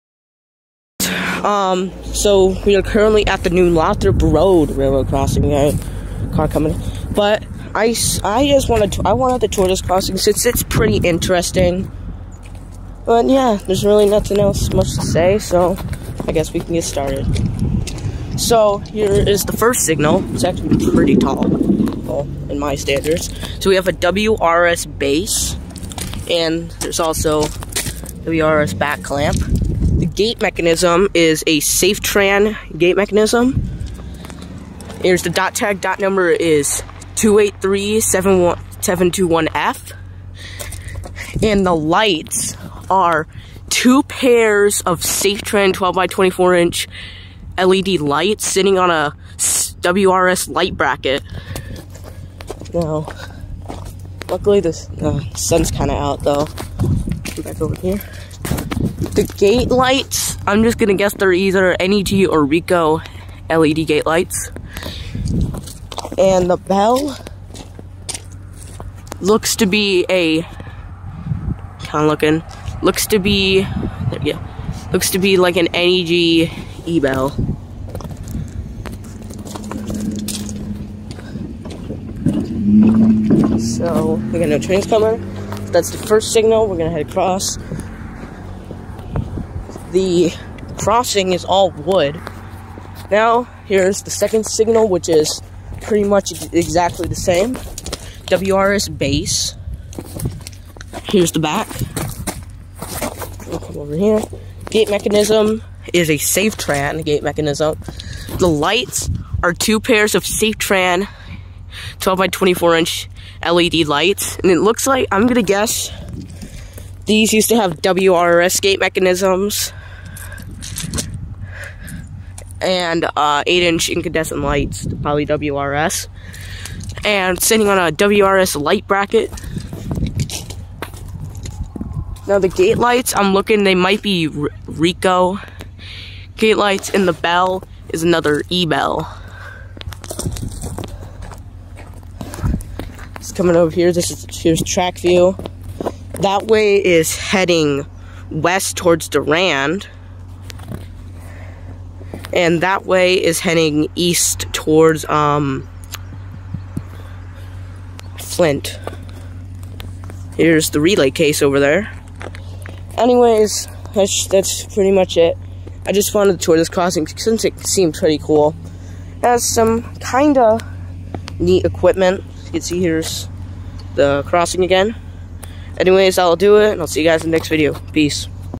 RR Crossing tour (New Lothrop Rd)
Nothing much to put here, just an average CN crossing.

Location: New Lothrop Rd, Durand Michigan

Owner: CN

Former Owner(s): GTW

Status: Active

Trains per day: unknown

Tracks: 2

Quiet Zone: No